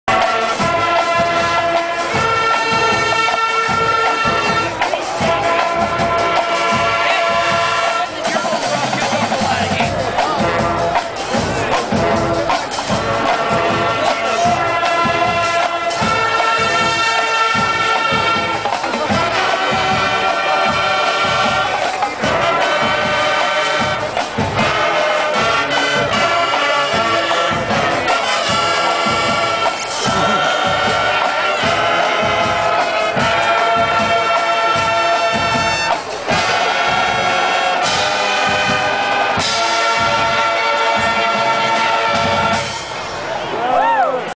November 21st, 2008 Toledo v. Miami (Oh) at the Glass Bowl
Dancing Rock-ets with redhead Janine cheering for Toledo!

Courtney's out with the Dancing Rock-ets shaking it out in the Glass Bowl!